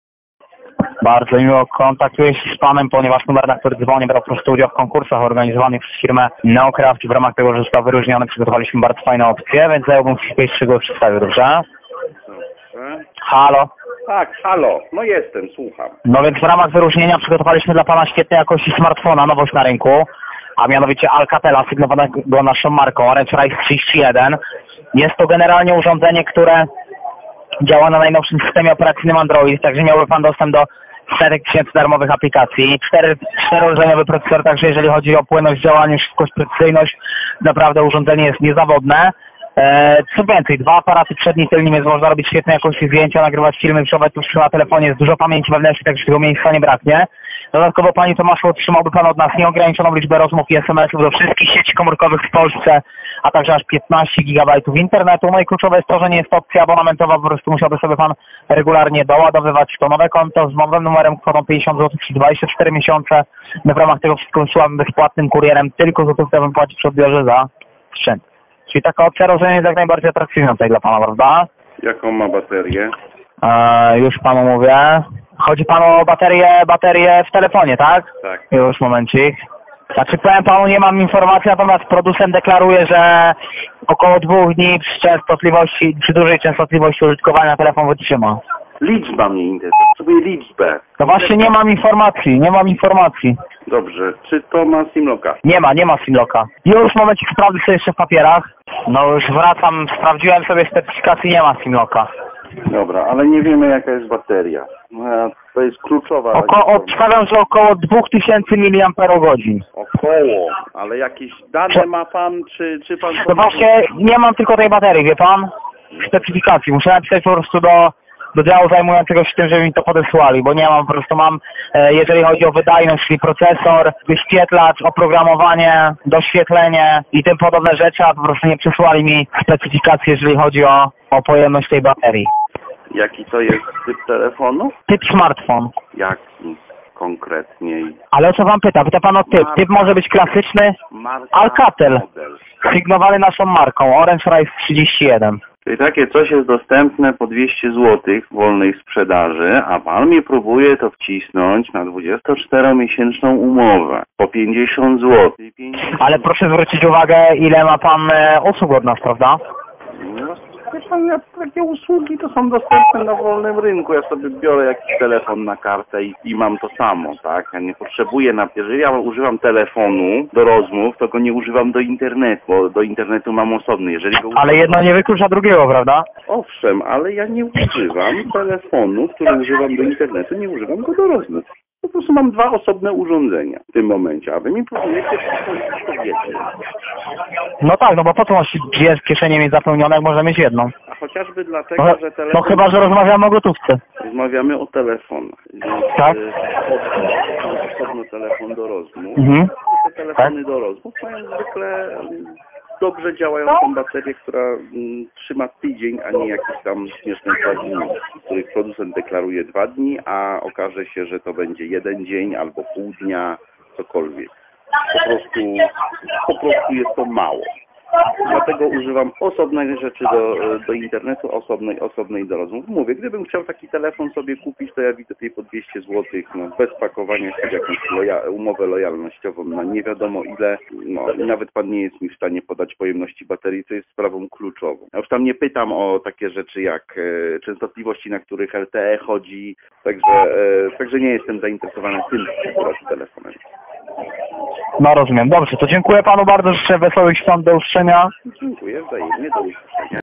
Orange Rise 31 i wciskanie ciemnoty przez telemarketerów
Rozmowa z konsultantem,który próbuje wcisnąć telefon wart 179zł w ramach 24 miesięcznej umowy po 50zł/miesięcznie,a na pytanie o pojemność baterii (która w w/w modelu jest śmieszna) nabiera wody w usta.Jeśli dzwoni do Ciebie telemarketer to znaczy,że oferowany przez niego produkt się nie sprzedaje sam i trzeba mu pomagać